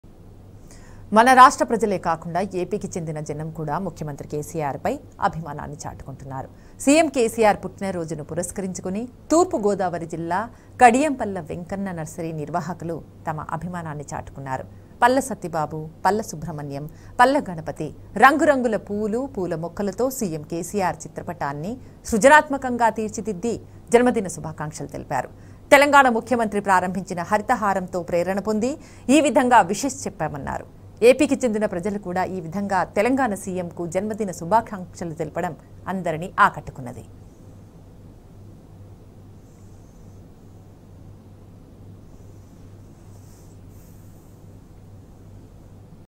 AP's East Godavari Man Conveyed Birthday Wishes To CM KCR In Unique Way | Novel Portrait | T News
The green-portrait is receiving accolades, especially from the netizens on social media for the way non-Telangana people has chosen to greet the CM with such affection and creativity.A Nursery plant owner from Kadiam in East Godavari district Palla Venkanna has found a novel way to exhibit his love for the Telangana Chief Minister Chandrasekhar Rao by drawing his picture through plant-Carving to commemorate his birthday on February 17.AP made a creative portrait of CM KCR using various coloured flowers and flower plants and conveyed their birthday wishes to the CM.